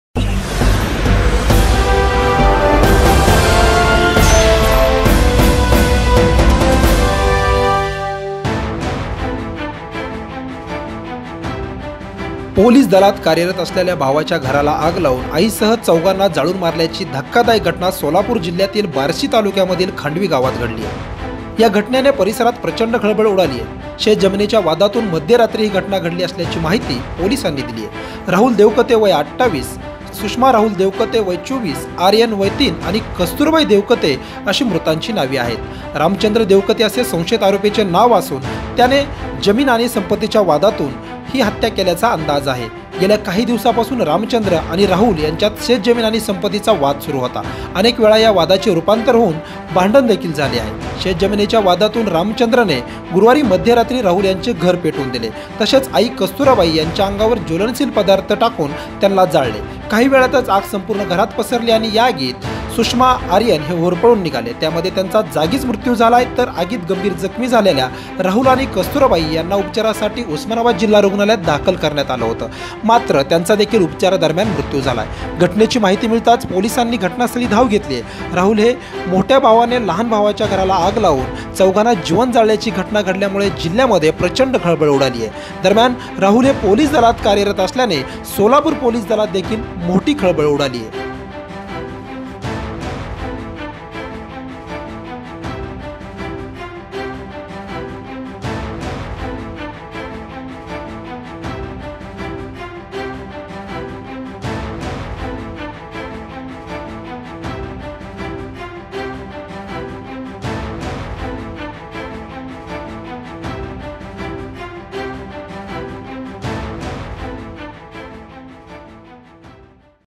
सोलापूर बार्शी : पोलिस असलेल्या भावाच्या घराला पेटवलं
पोलिस दलात कार्यरत असलेल्या भावाच्या घराला आग लावून आईसह चौघांना  मारल्याची घटना सोलापूर जिल्हयातील बार्शी तालुक्यातील खांडवी गावात घडल्याने प्रचंड खळबळ उडाली आहे. घटनेची माहिती मिळताच पोलिसांनी घटनास्थळी धाव घेतली.